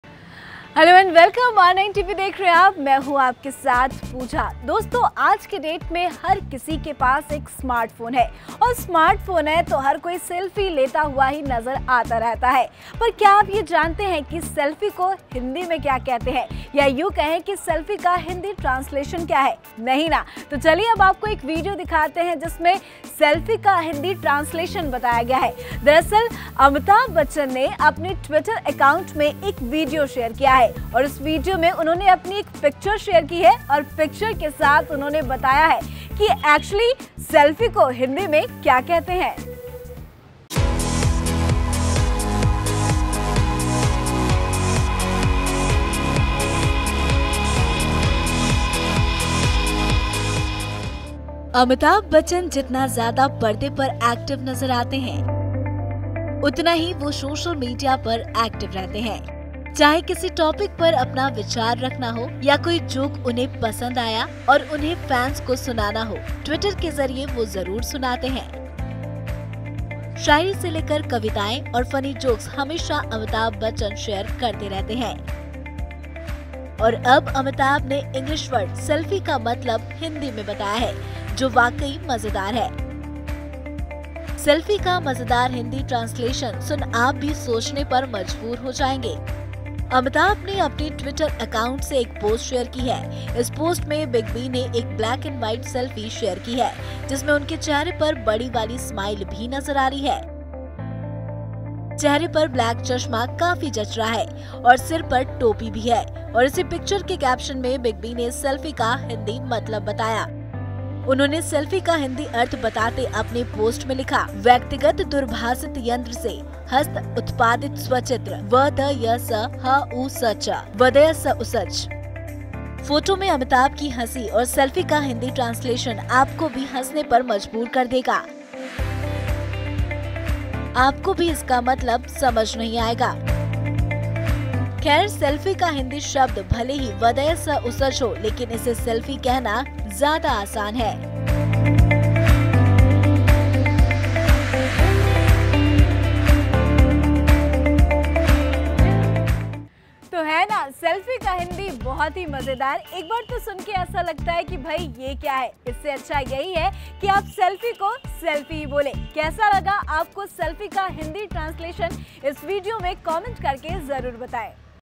BIG B ने ट्वीट कर बताया सेल्फी का हिंदी वर्जन
अमिताभ बच्चन ने ट्वीट कर सेल्फी का हिंदी में मतलब समझा दिया है...आप भी देखिये सेल्फी का हिंदी वर्जन क्या है….